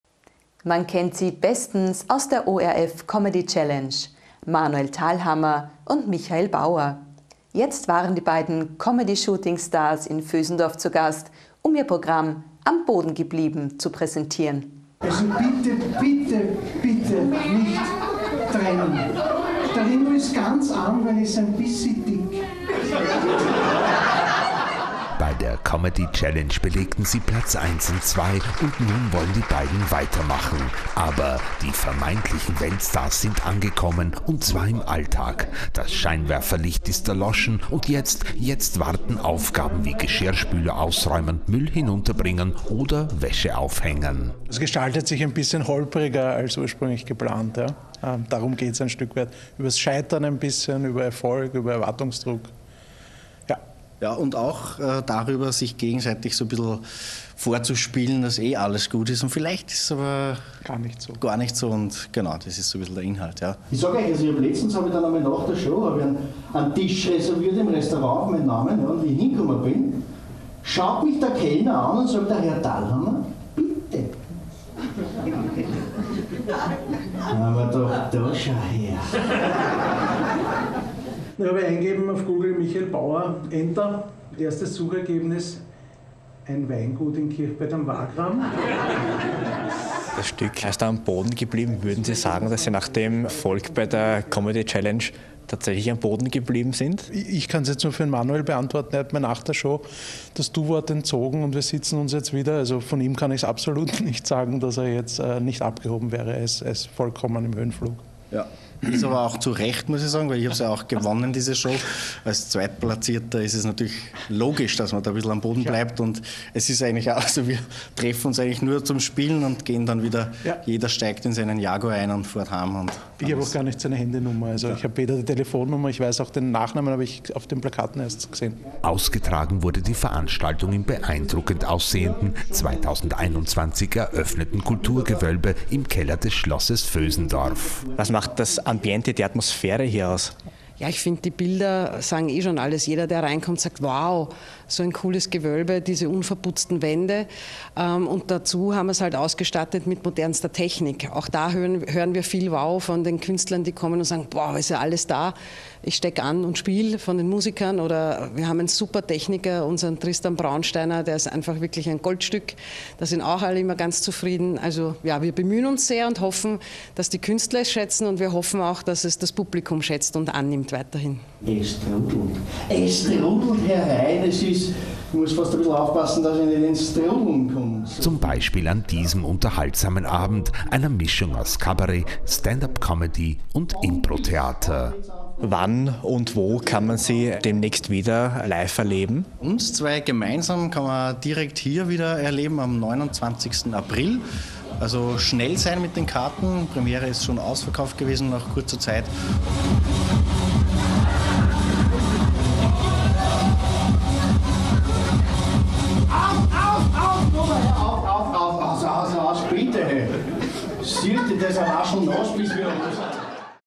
Kabarett in Vösendorf
Man kennt sie bestens aus der ORF Comedy Challenge: Manuel Thalhammer und Michael Bauer. Jetzt waren die beiden Comedy-Shootingstars in Vösendorf zu Gast, um ihr Programm „Am Boden geblieben“ zu präsentieren.